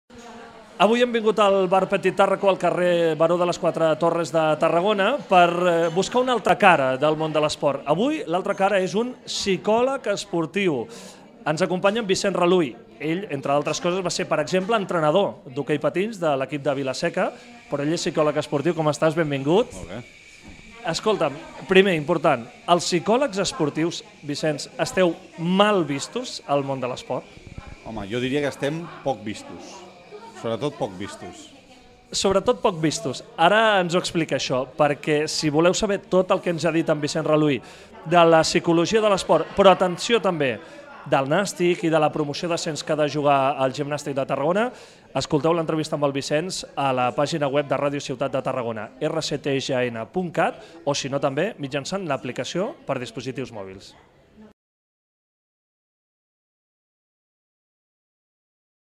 Converses amb Carles Cortés... Vicenç Raluy (psicòleg esportiu)
Vicenç Raluy és entrenador i psicòleg esportiu. Ens hem citat amb ell al bar Petit Tarraco (c/. Baró de les Quatre Torres) per parlar de la psicologia aplicada als equips i esportistes i per aprofunciar en el cas del Nàstic a les portes de l’eliminatòria d’ascens contra el Huesca.